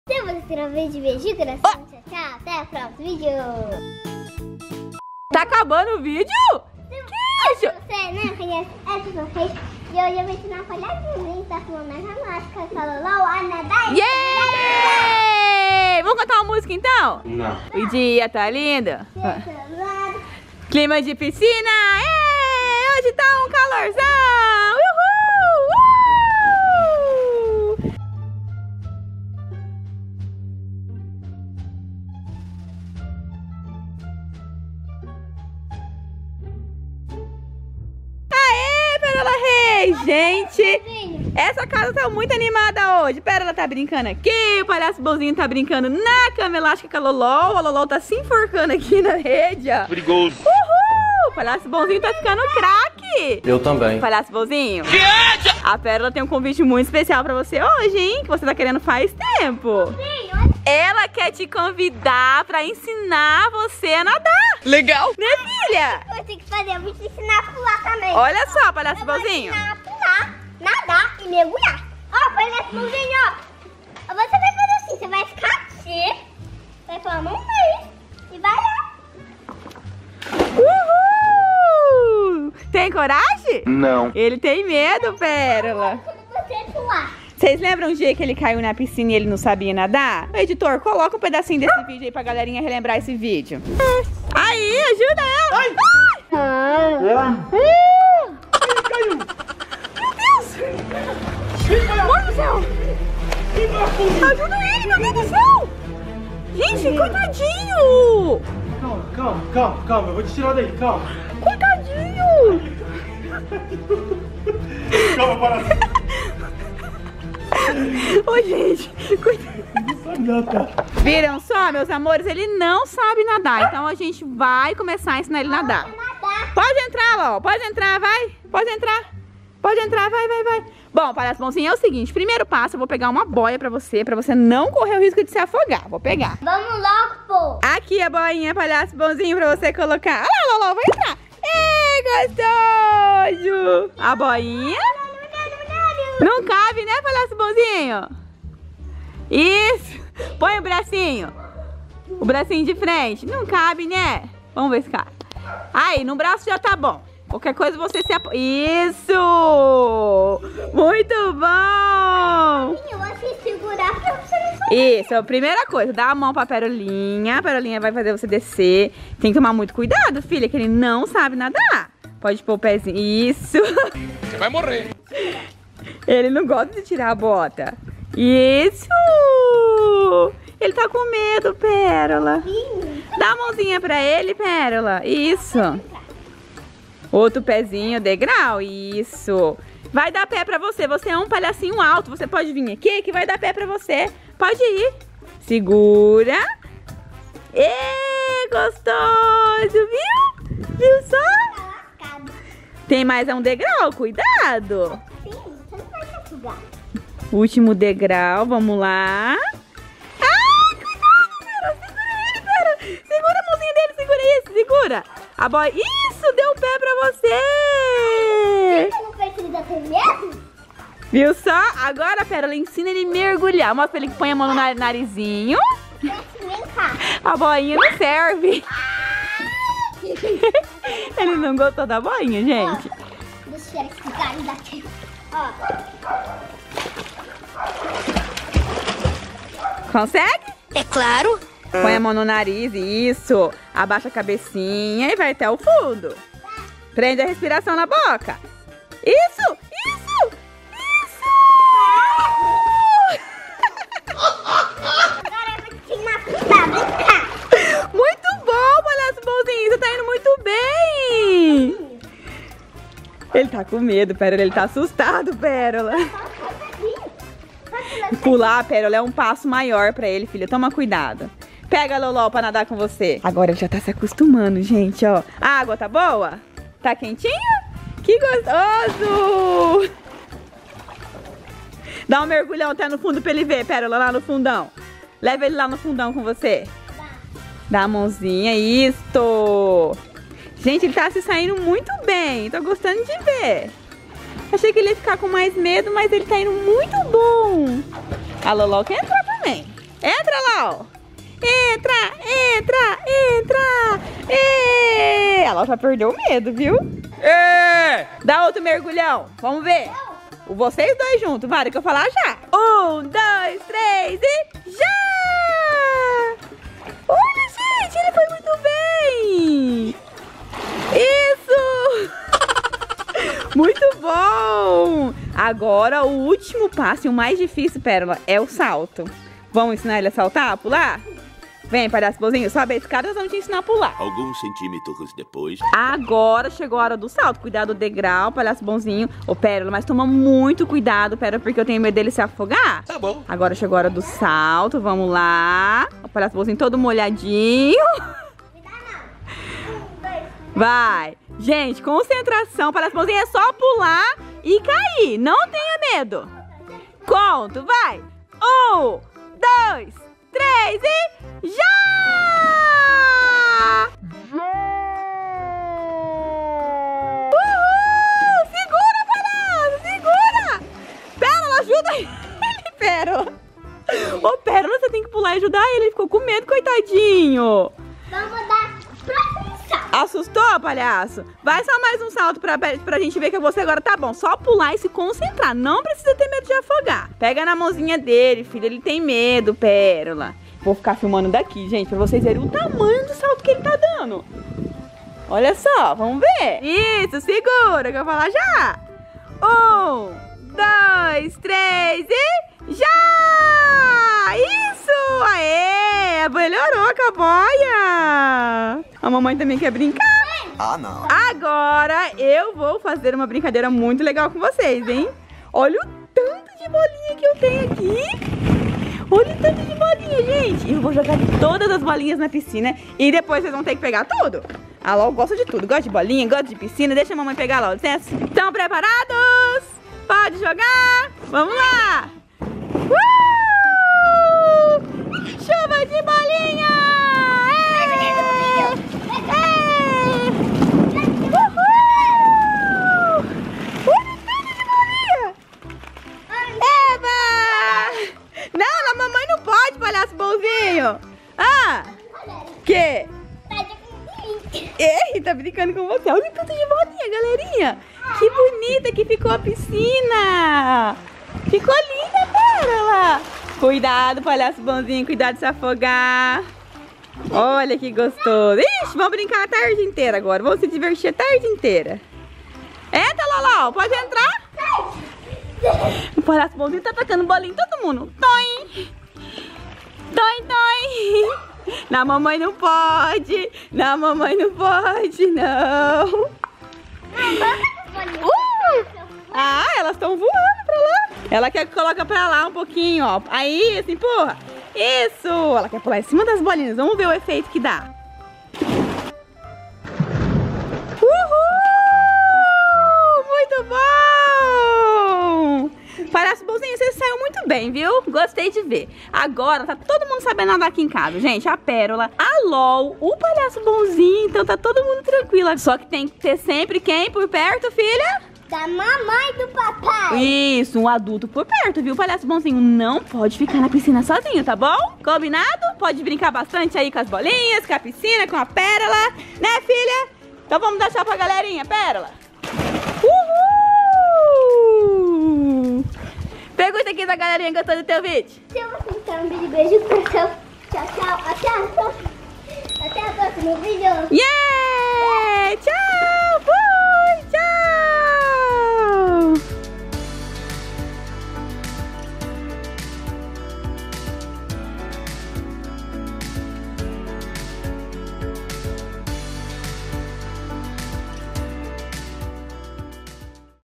ENSINAMOS O PALHAÇO 🤡 BONZINHO A NADAR
GENTE HOJE VOU TENTAR ENSINAR MEU AMIGUINHO PALHAÇO BONZINHO A NADAR
👑 PEROLA REIS 👑
✔️ CANAL DE ENTRETENIMENTO